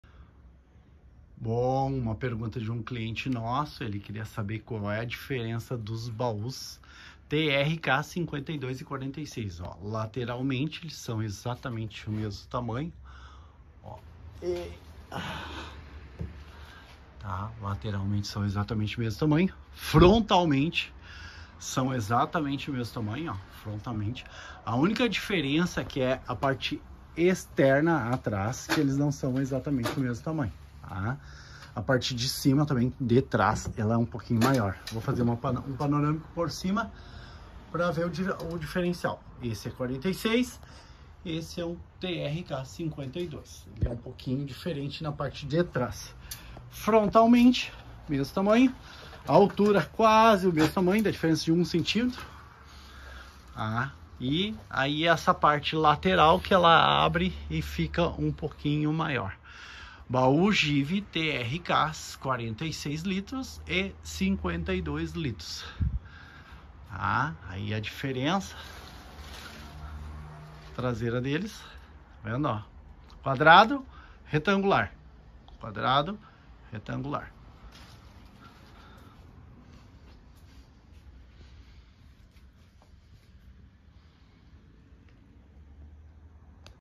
Diferença Baús GIVI trk 46 x Trk 52
Mostrando a diferença entre baús GIVI TRK 46 e TRK 52.